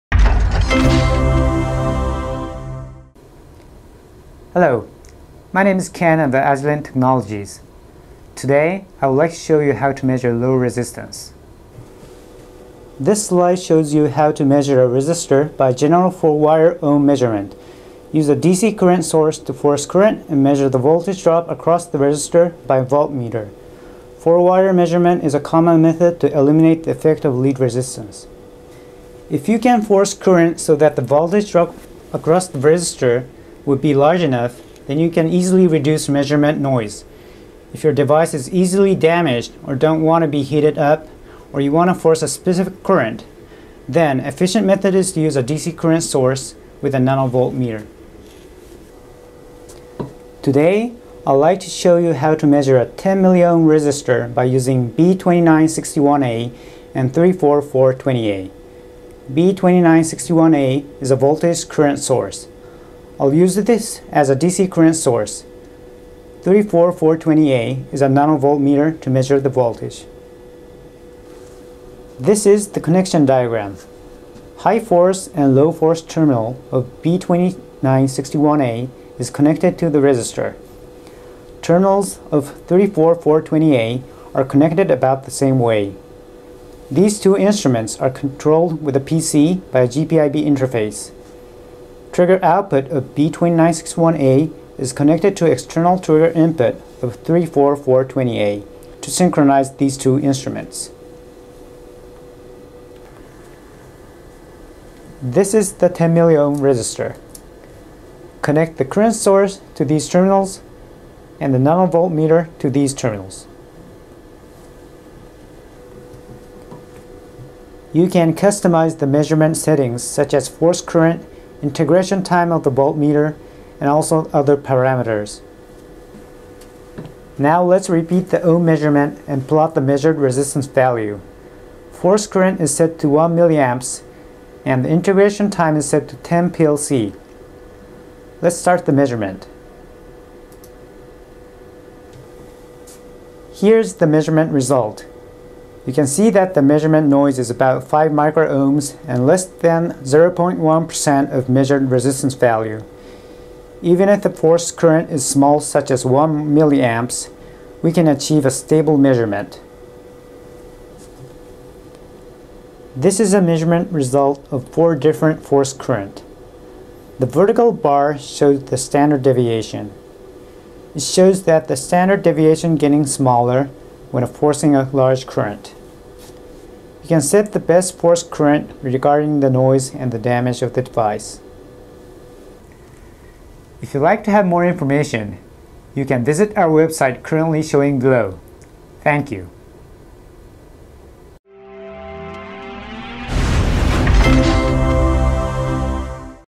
How to measure low resistance -1 : Optimizing bias current for milliohm level - B2960 - BEMT#8
Introducing how to measure milliohm level low resistance. The DMM with 4-wire connection is popular to measure the resistance. In order to reduce the measurement noise, higher current is used to have larger voltage drop across the resister to reduce the noise. If the DUT is easily damaged or specific current is required, the combination of current source and nano-volt meter is used.